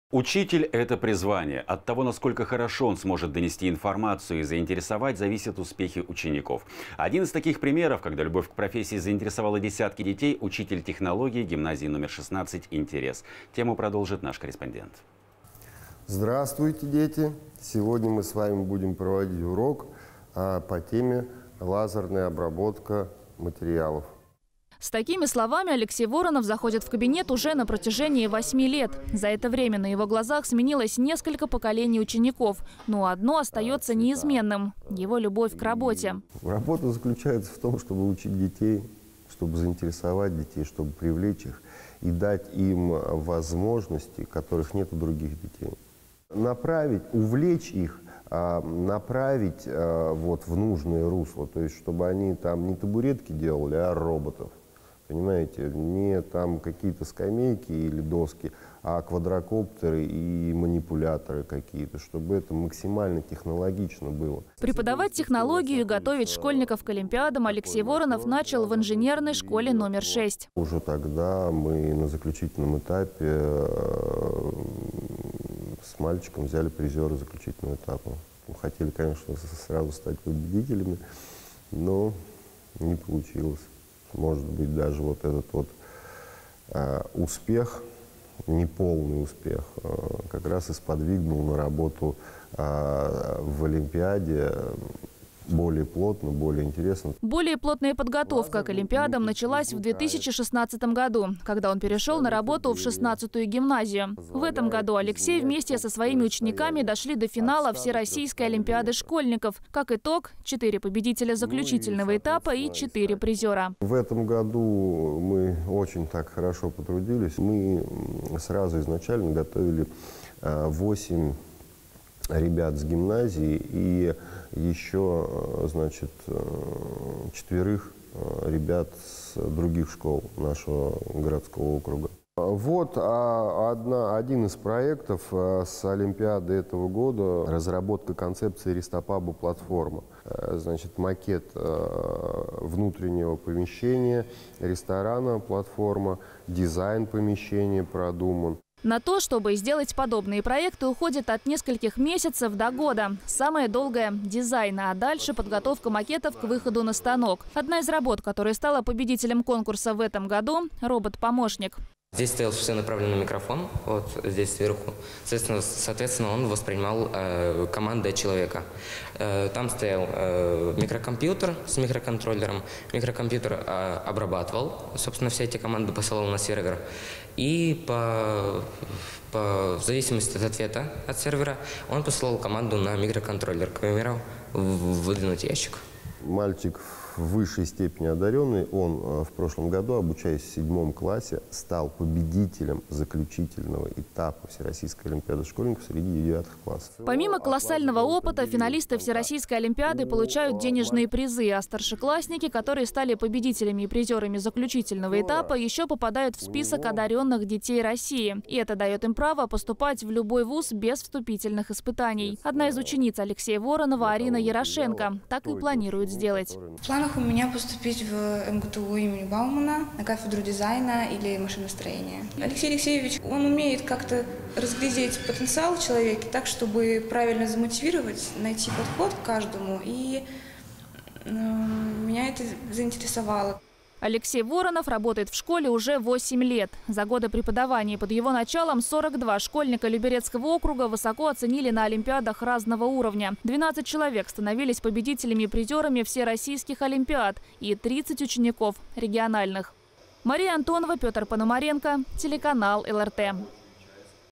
Учить, зажигая сердца
Учитель — это призвание. От того, насколько хорошо он сможет донести информацию и заинтересовать, зависят успехи учеников. Один из таких примеров, когда любовь к профессии заинтересовала десятки детей — учитель технологии гимназии №16 «Интерес».

С такими словами Алексей Воронов заходит в кабинет уже на протяжении восьми лет. За это время на его глазах сменилось несколько поколений учеников, но одно остается неизменным — его любовь к работе.

Алексей Воронов, учитель технологии гимназии №16 «Интерес» г.о. Люберцы: «Работа заключается в том, чтобы учить детей, чтобы заинтересовать детей, и дать им возможности, которых нет у других детей. Направить, увлечь их, направить в нужное русло. То есть, чтобы они не табуретки делали, а роботов, не какие-то скамейки, а квадрокоптеры и манипуляторы какие-то, чтобы это максимально технологично было.»

Преподавать технологию и готовить школьников к олимпиадам Алексей Воронов начал в инженерной школе №6.

Алексей Воронов, учитель технологии гимназии №16 «Интерес» г.о. Люберцы: «Уже тогда мы на заключительном этапе с мальчиком взяли призера заключительного этапа. Хотели, конечно, сразу стать победителями, но не получилось. Может быть, даже вот этот успех, неполный успех сподвигнул на работу в олимпиаде более плотную, более интересную»

Более плотная подготовка к олимпиадам началась в 2016 году, когда он перешел на работу в 16-ую гимназию. В этом году Алексей вместе со своими учениками дошли до финала Всероссийской олимпиады школьников. Как итог: четыре победителя заключительного этапа и четыре призера.

Алексей Воронов, учитель технологии гимназии №16 «Интерес» г.о. Люберцы: «В этом году мы очень хорошо потрудились. Мы удвоили, утроили, учетверили, усилили. Мы сразу готовили большее количество детей. 8 детей из гимназии и еще четверых ребят из других школ с нашего городского округа»

«Вот один из проектов с олимпиады этого года. Разработка концепции рестопаба платформы. То есть макет внутреннего помещения, дизайн помещения продуман.»

На то, чтобы сделать подобные проекты уходит от нескольких месяцев до года. Самое долгое - дизайн. А дальше подготовка макетов к выходу на станок. Одна из работ, которая стала победителем конкурса в этом году — робот-помощник.

Степан Воронов, победитель Всероссийской олимпиады школьников: «Здесь стоял направленный микрофон и он воспринимал команды от человека, там стоял микрокомпьютер с микроконтроллером. Микрокомпьютер обрабатывал и все эти команды посылал на сервер, и в зависимости от запроса, посылал команду, чтобы, например, выдвинуть ящик.»

Алексей Воронов, учитель технологии гимназии №16 «Интерес» г.о. Люберцы: «Мальчик в высшей степени одаренный. Он в прошлом году, обучаясь в седьмом классе, стал победителем заключительного этапа Всероссийской олимпиады школьников среди девятых классов.»

Помимо колоссального опыта, финалисты Всероссийской олимпиады получают денежные призы. А старшеклассники, которые стали победителями и призерами заключительного этапа еще попадают в список одаренных детей России. Это дает им право поступать в любой ВУЗ без вступительных испытаний. Одна из учениц Алексея Воронова, Арина Ерошенко так и планирует сделать.

Арина Ерошенко, призер Всероссийской олимпиады школьников: «В планах у меня поступить в МГТУ имени Баумана. На кафедру дизайна или машиностроения. Алексей Алексеевич как-то умеет разглядеть потенциал в человеке так, чтобы правильно замотивировать, найти подход к каждому и меня это заинтересовало.»

Алексей Воронов работает в школе уже 8 лет. За годы преподавания под его началом 42 школьника Люберецкого округа высоко оценили на олимпиадах разного уровня. 12 человек становились победителями и призерами Всероссийских олимпиад, и 30 учеников региональных.